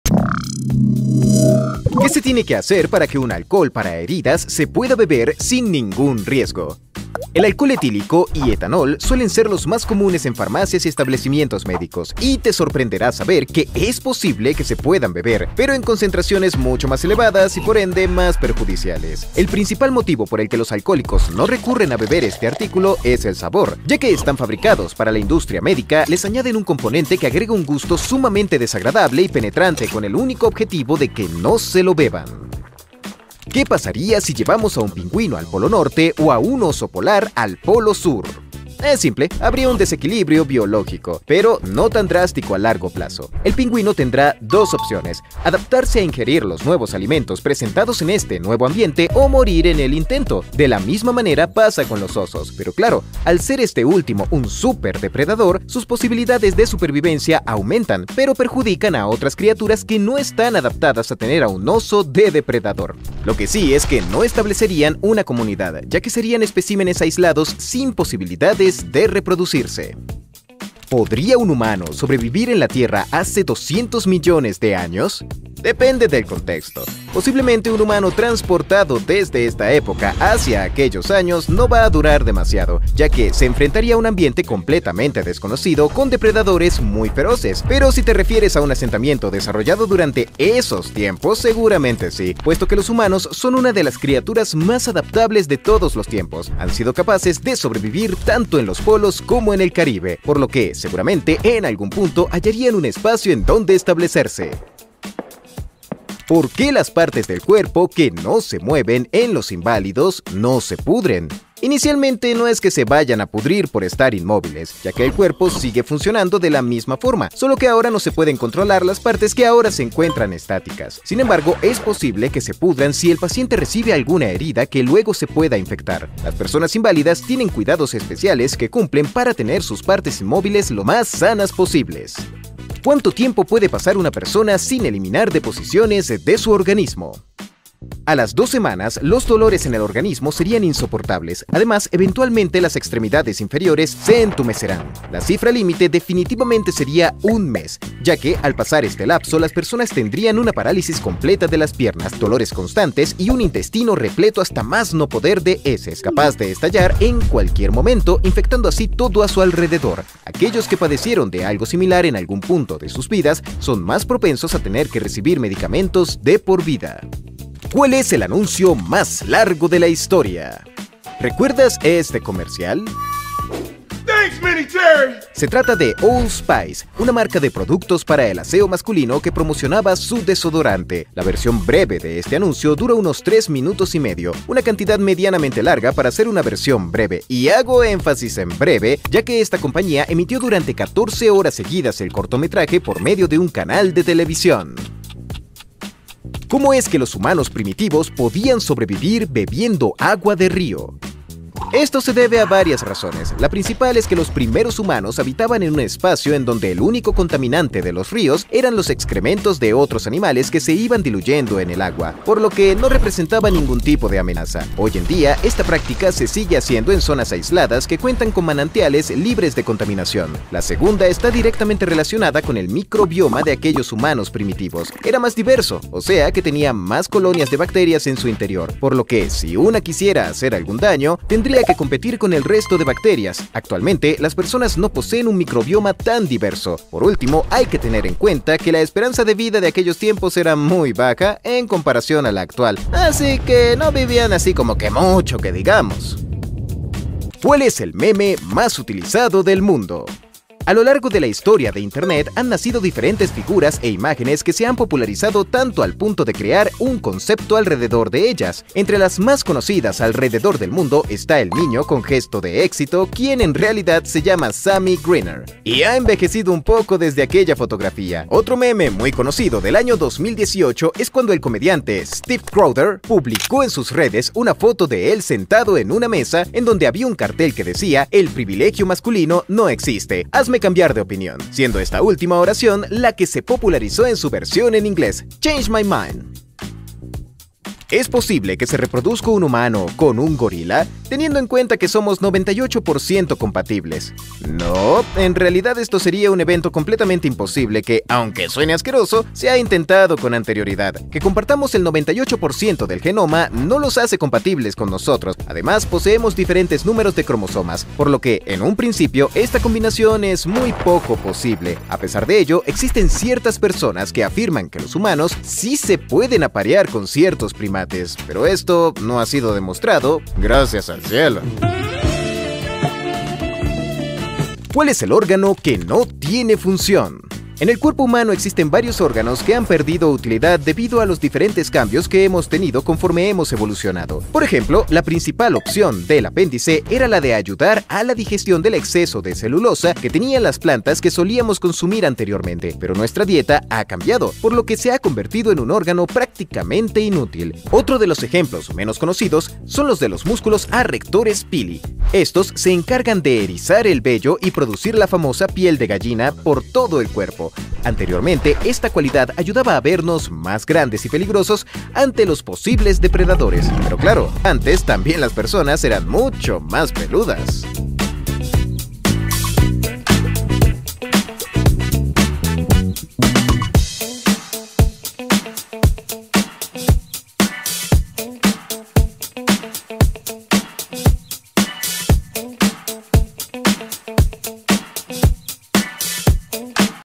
Respondiendo 10 Preguntas Tontas que TODOS nos hicimos 🤯🔥
Estas son algunas preguntas que todos alguna vez nos hicimos PARTE 12 📢😵
Reponiendo preguntas curiosas

Preguntas curiosas